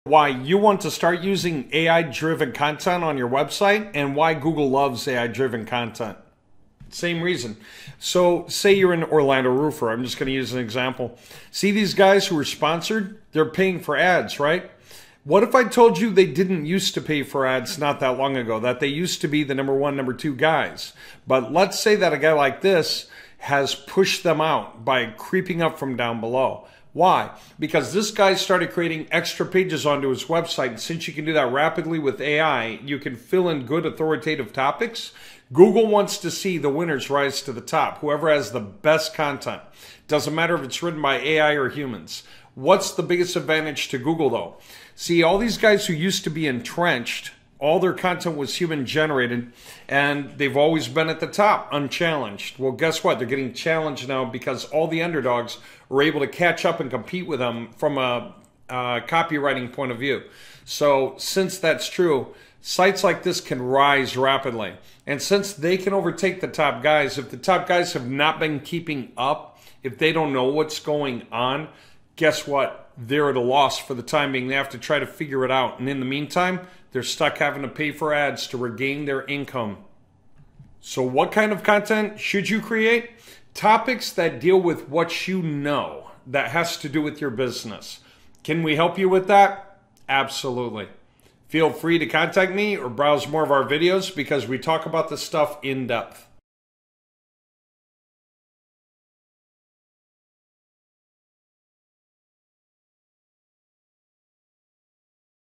Biggest Reason to Start Using AI on Your Websites (Now)
If you're an underdog, using AI now will help you rise to the top on keywords. As you do that, the entrenched leaders will stop dropping - and have to pay for ads to maintain cashflow! That's also great for you, since you can develop a stronger budget than them. It's not just about CATCHING UP to your competitors... but instead, also BEATING them at the same time!

Best Regards, Dave Bennett (maxedtraffic)